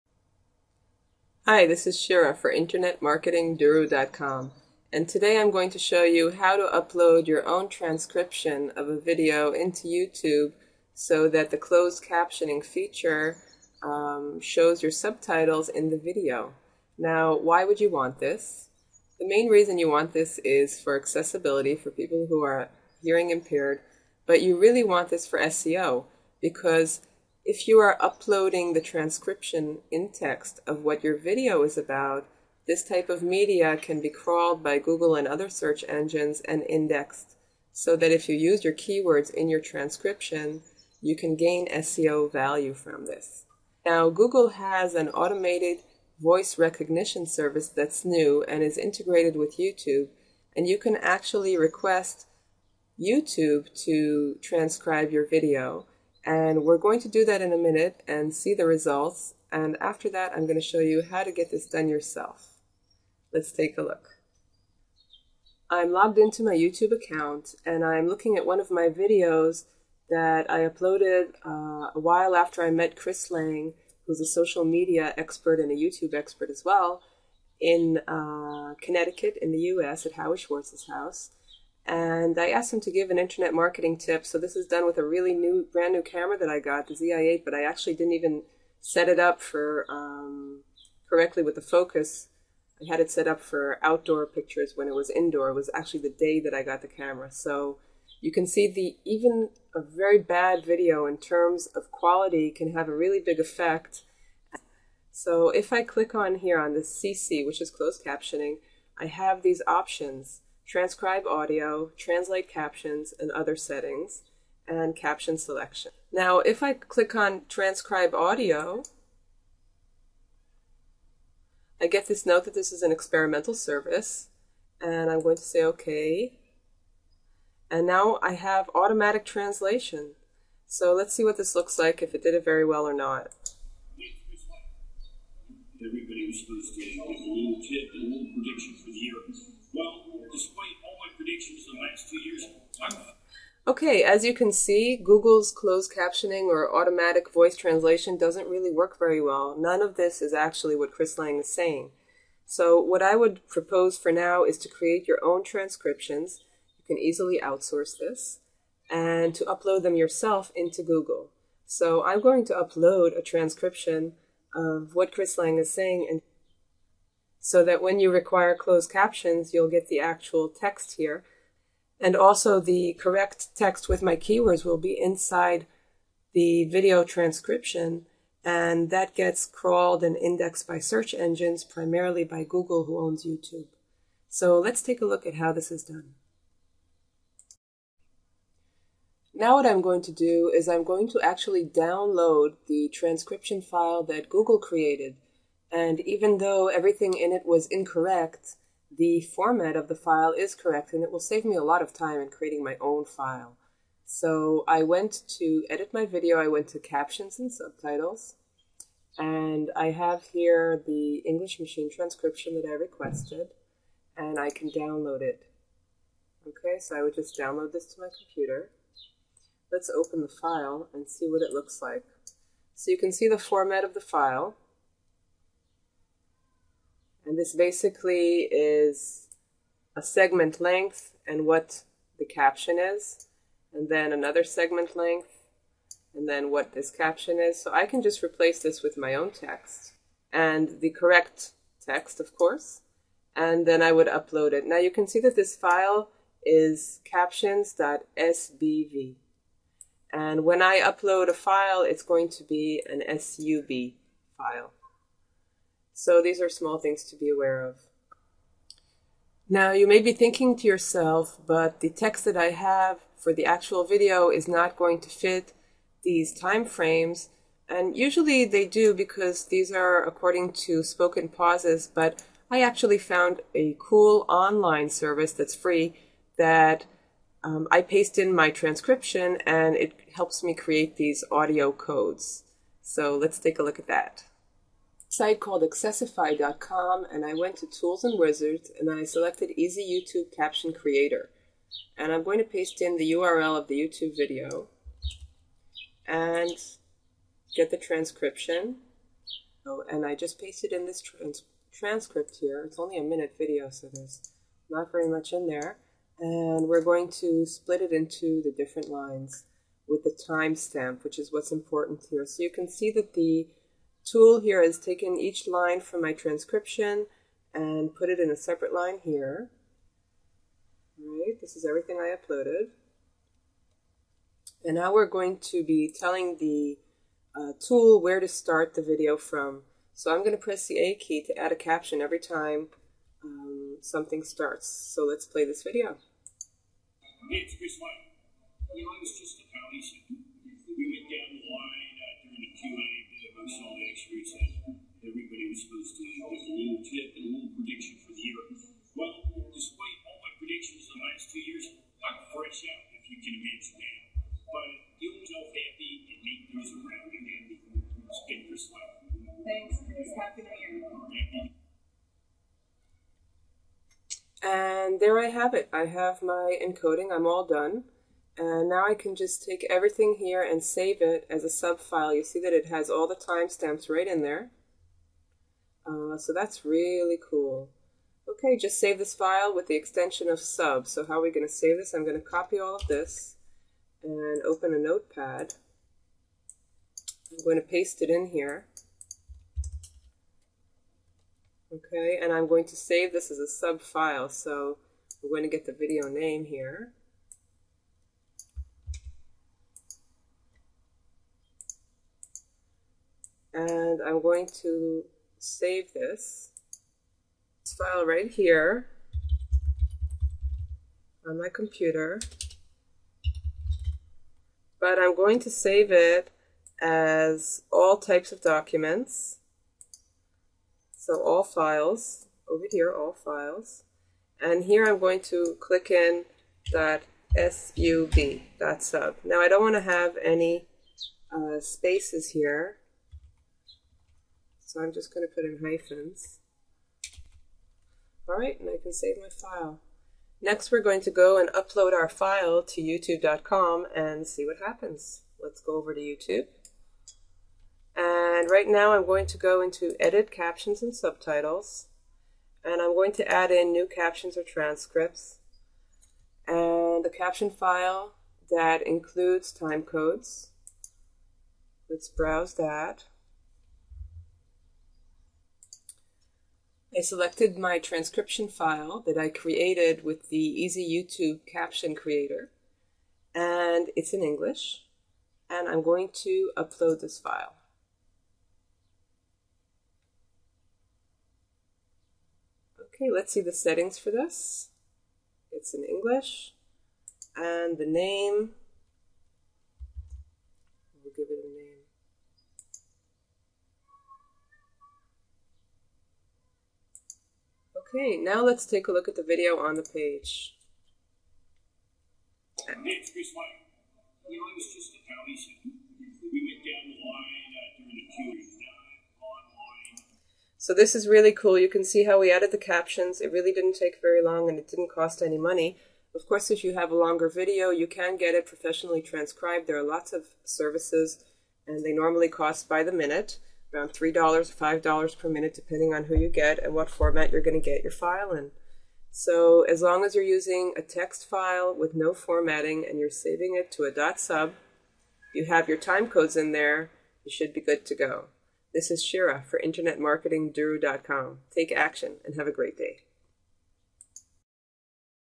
Create and add Time Coded Transcriptions for YouTube Closed Captioning
In this video Internet Marketing Duru shows you how to add a closed caption subtitles and transcript to your YouTube Video.
How to get a YouTube compatible time coded transcript file for free, and how to leverage your YouTube transcript for SEO value.

Take action and have a great day!
Duru